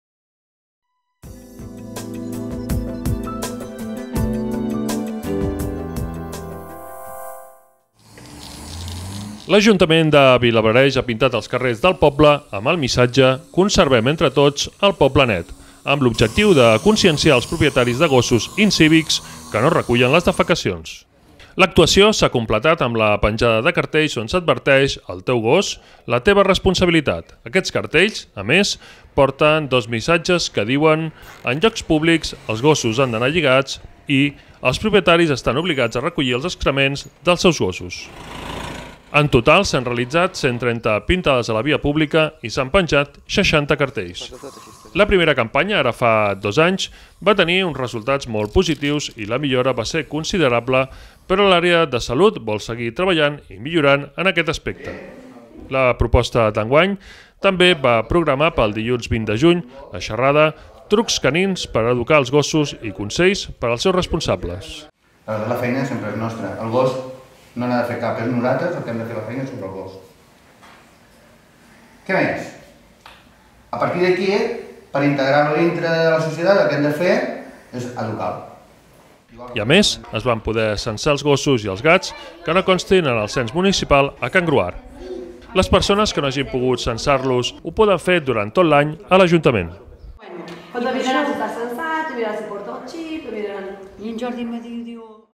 Campanya "Conservem entre tots el poble net"
L’Ajuntament de Vilablareix ha pintat els carrers del poble amb el missatge “Conservem entre tots el poble net” amb l’objectiu de conscienciar als propietaris de gossos incívics que no recullen les defecacions.